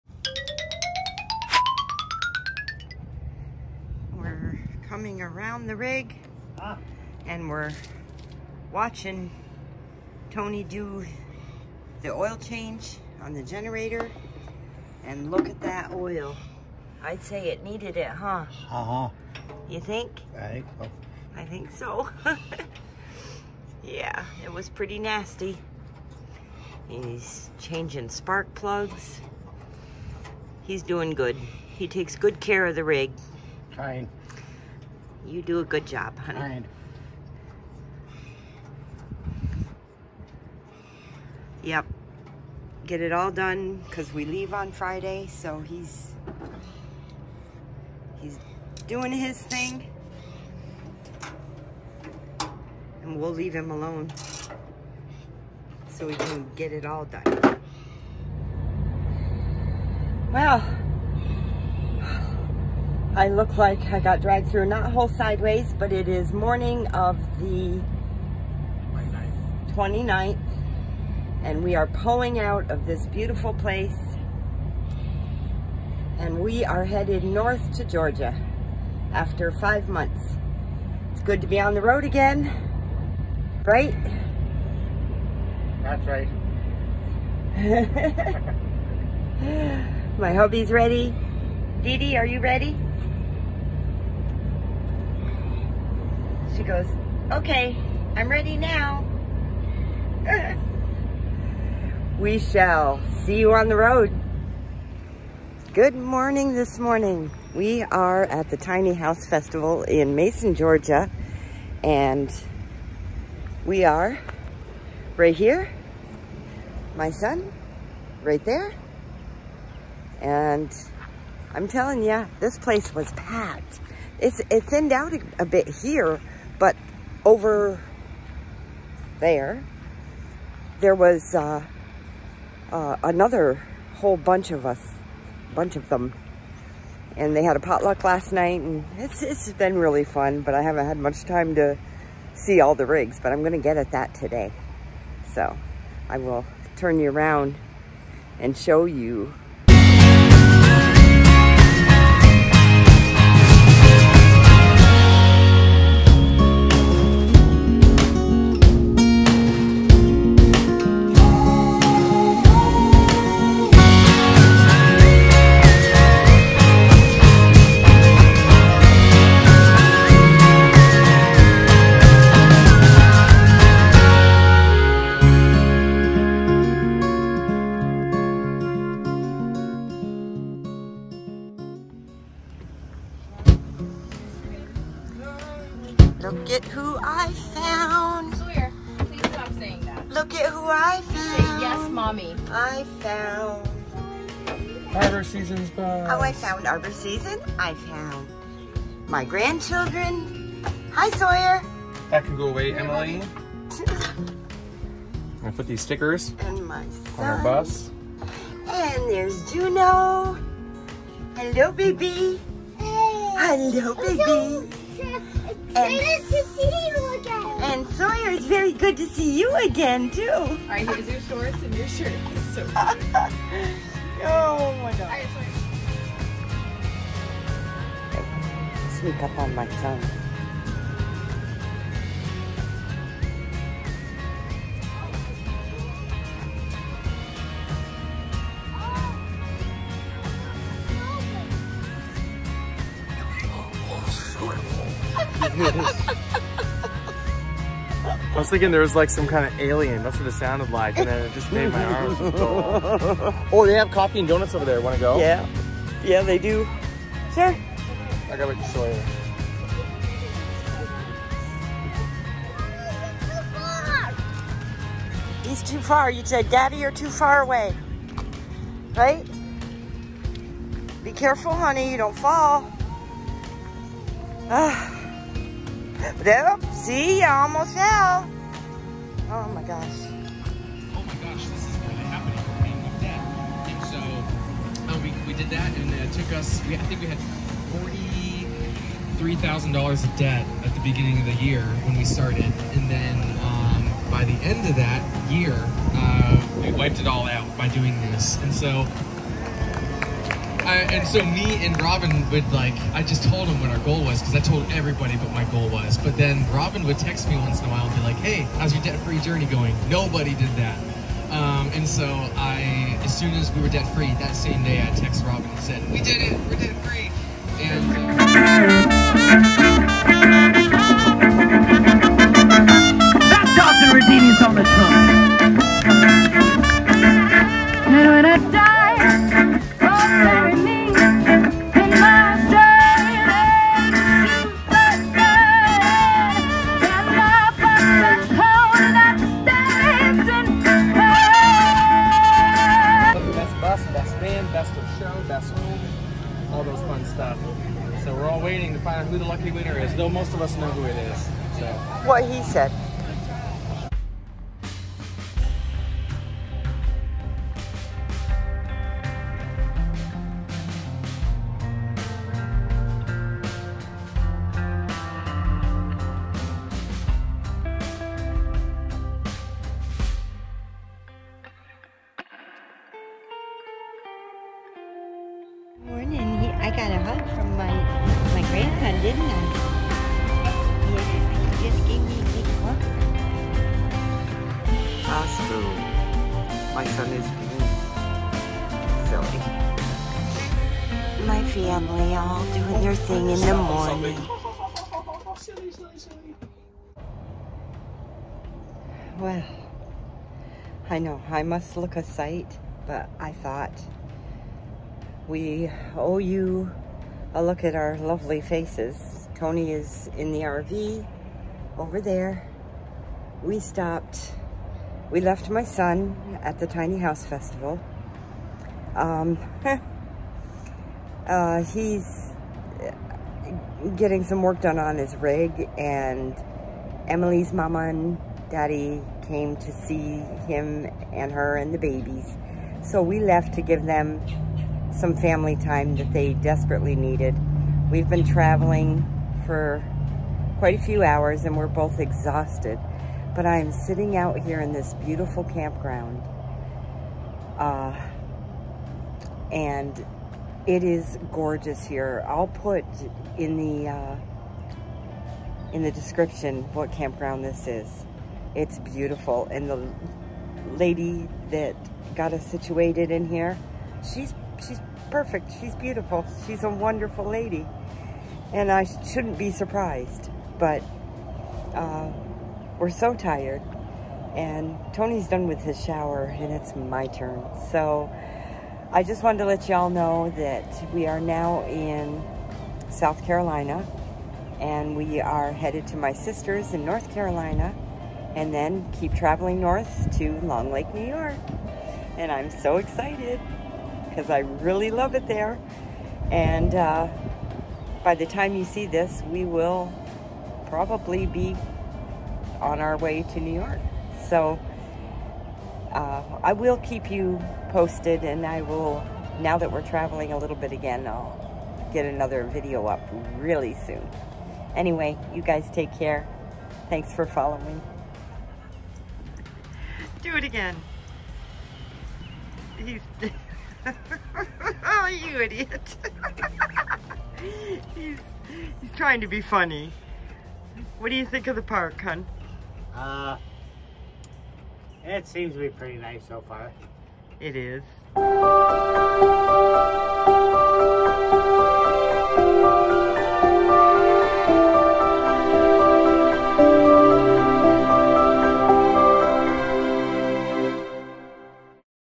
Tiny House Festival and Arbour Season
We travel to Georgia to the Tiny House Festival! Great time with family! Then we stopped at Columbia Camden RV Park In Lugoff, SC.  Beautiful park and Lana is the perfect hostess… new friend!❤️❤️❤️